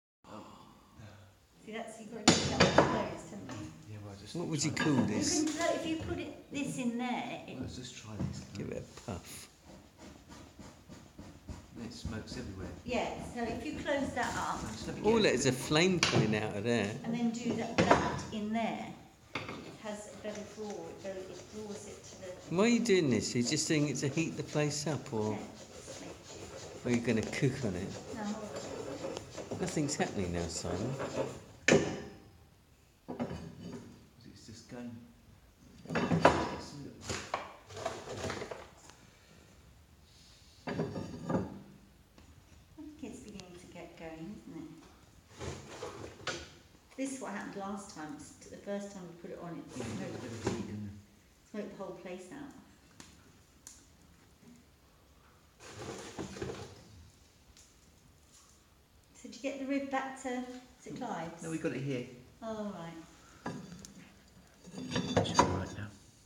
Ancient Spanish iron stove & oven in action
I guess this is the Spanish equivalent of an Aga or Rayburn.